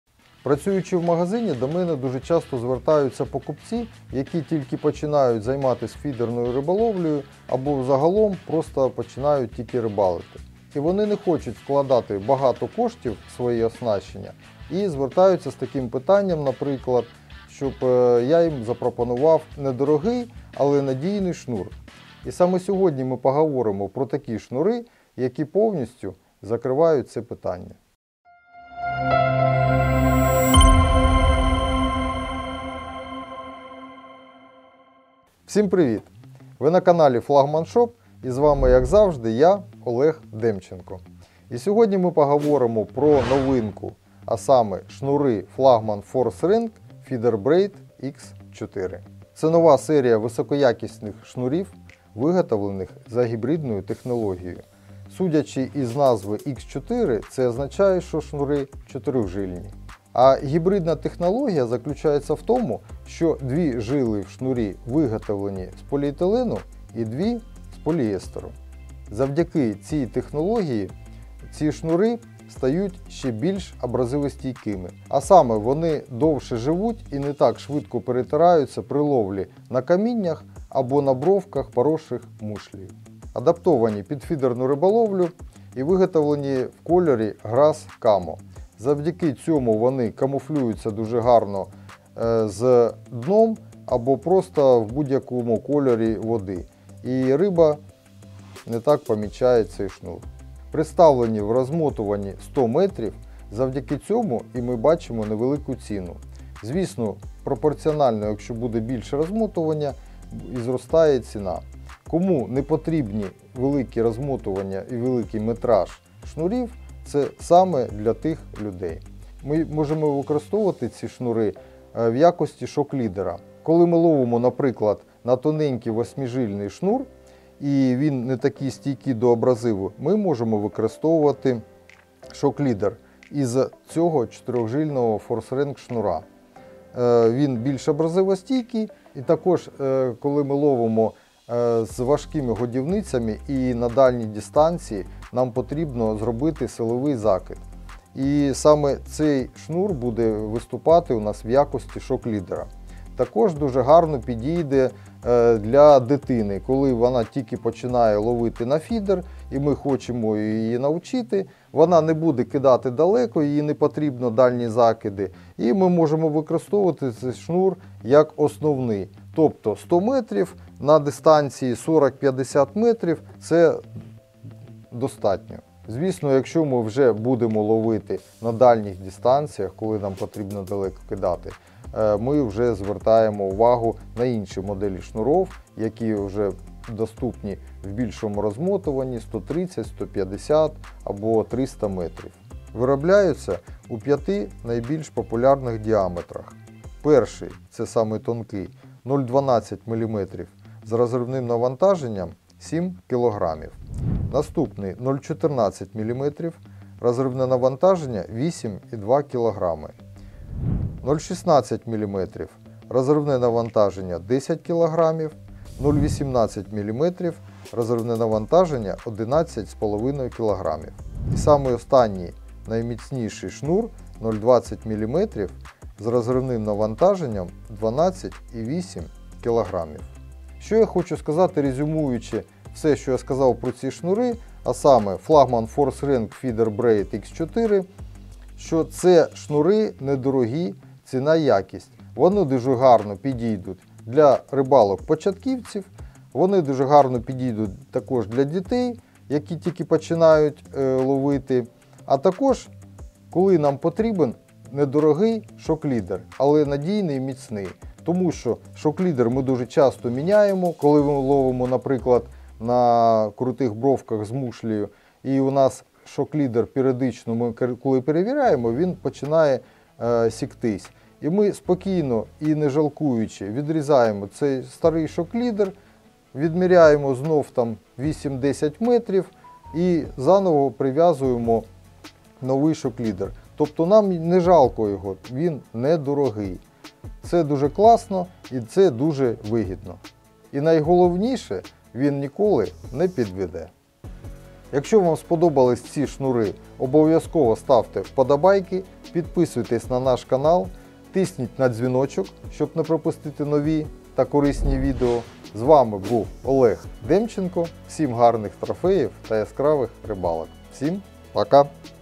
⚡️ Недорогі, але НАДІЙНІ шнури Flagman Force Rank Feeder Braid x4
Flagman Force Rank Feeder Braid Х4 – високоякісний PE-шнур нового покоління, виготовлений за гібридною технологією чотирижильного плетіння. Має високу абразивостійкість, здатний гідно протистояти камінню та гострим мушлям молюсків. Спеціальне покриття поверхні шнура робить його досить гладеньким, що дає змогу збільшити дальність і точність закидань.

Особлива технологія забезпечує шнурам Flagman Force Rank Feeder Braid Х4 високу щільність плетіння та мінімальну розтяжність, ідеальним чином підвищуючи чутливість снасті. Цей шнур адаптовано для фідерної ловлі та виготовлено в камуфляжному забарвленні Grass Camo, що дає змогу йому чудово зливатися з будь-яким ґрунтом і кольором води, залишаючись непомітним для риби. Шнури серії Force Rank Feeder Braid Х4 створені для широкого кола риболовів, мають чудові показники міцності, довговічності та найкраще у своєму сегменті співвідношення ціна/якість. Виробляються в розмотуванні 100 м і найбільш популярних діаметрах.

НАВІГАЦІЯ:

00:00 Початок
01:00 Особливості виготовлення та використання шнурів Flagman Force Rank
03:00 Діаметри шнурів Flagman Force Rank
03:50 Висновки